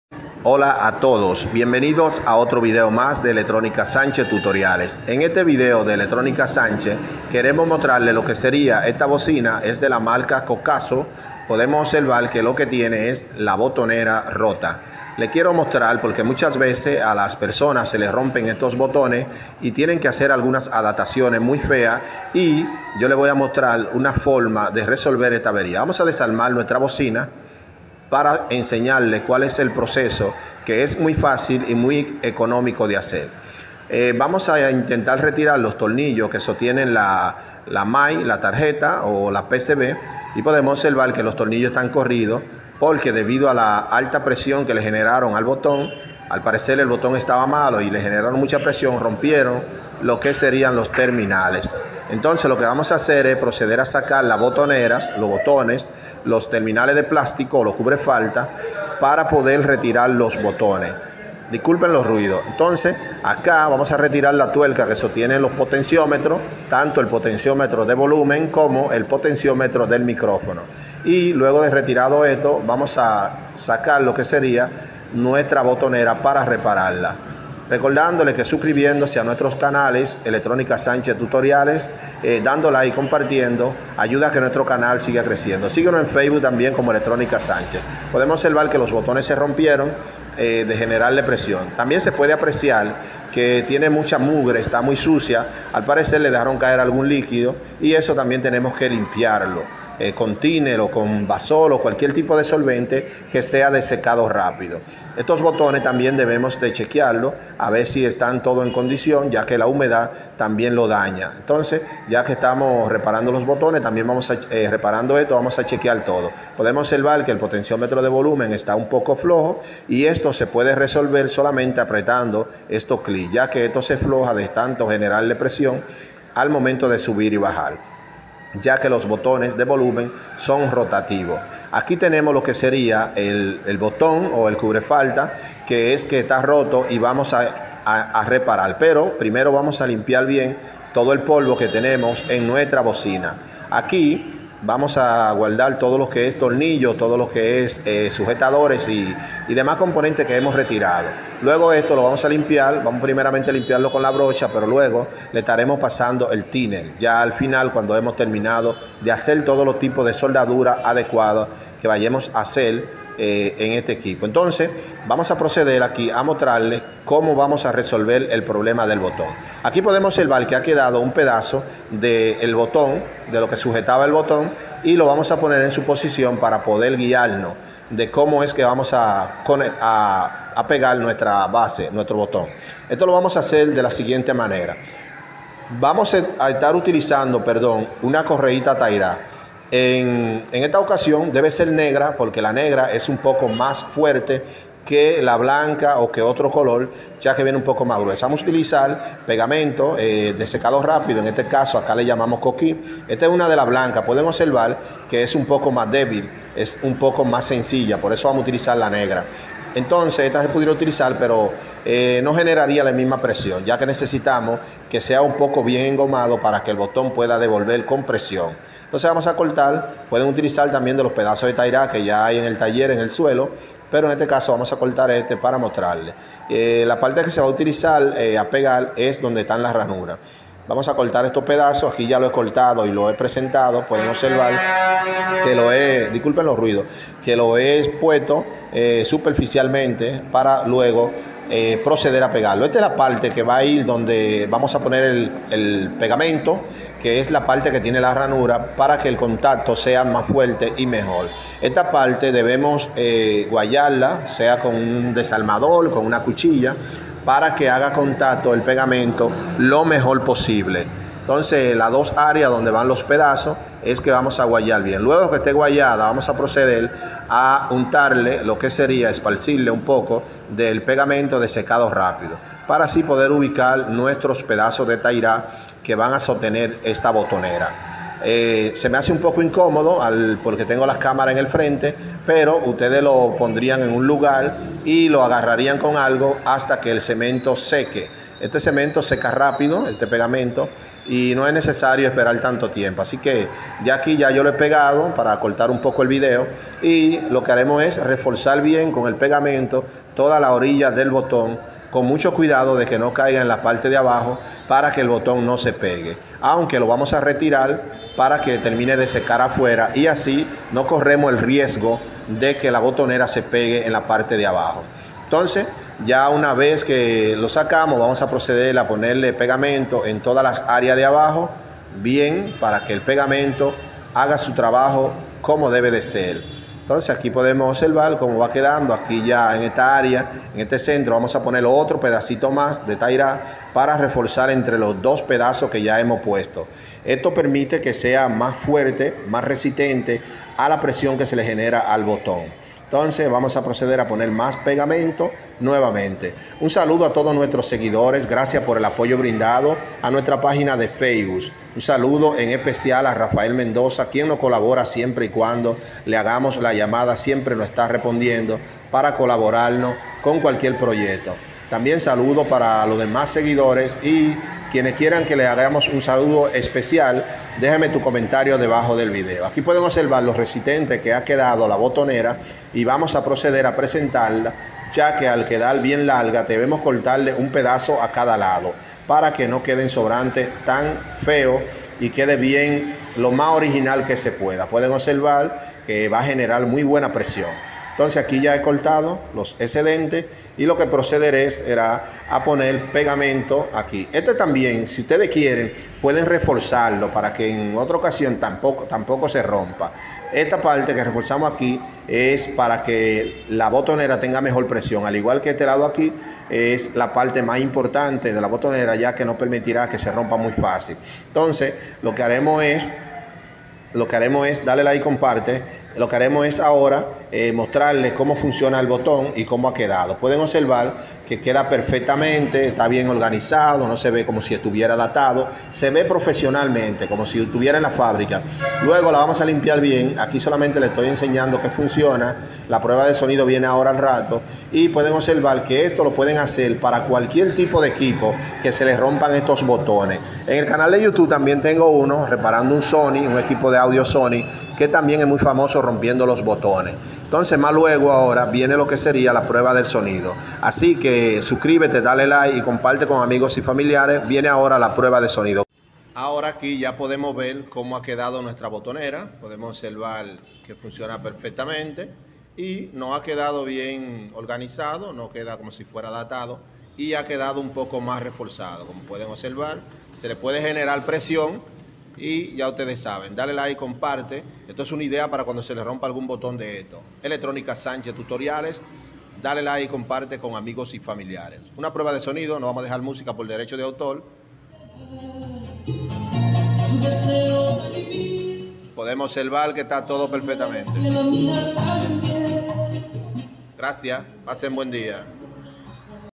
Hola aquí le mostraré cómo restaurar un botón roto en bocina amplificadas
hola a todos ustedes aquí le mostraré cómo reparar un botón roto en bocina recargable amplificadas y está técnica de restauración de botones se puede aplicar en amplificadores de audio con problema en los botones del panel frontal gracias